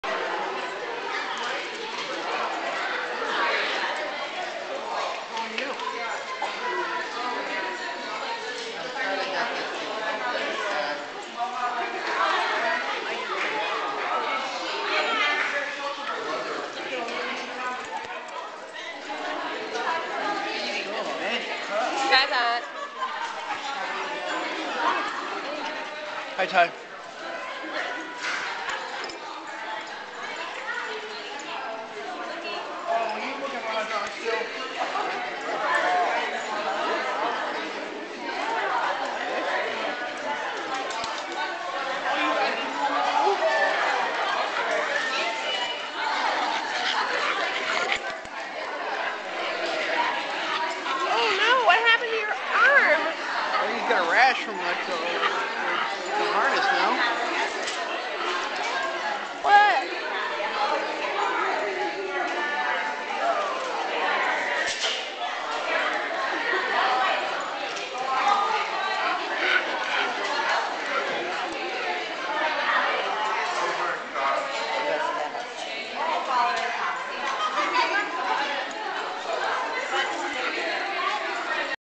Pugs Galore at the Los Angeles Pug Meetup
Here is the April Meetup for the Los Angeles & Hollywood Pug group. It was a lot of fun! We always have a good time at The Zoom Room.